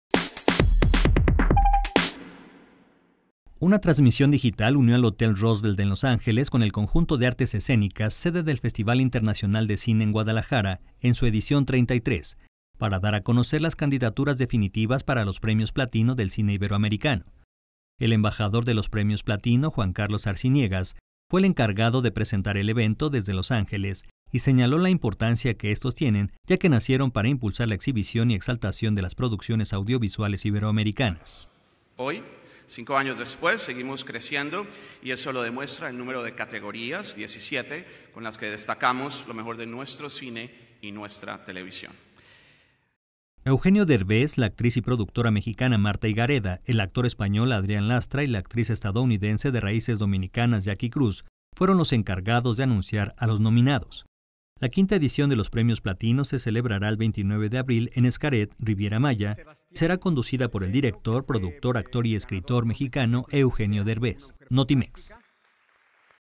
Revelan nominados a los Premios Platino de Cine Iberoamericano
Eugenio Derbez, la actriz y productora mexicana Martha Higareda, el actor español Adrián Lastra y la actriz estadounidense de raíces dominicanas Jackie Cruz fueron los encargados de anunciar a los nominados.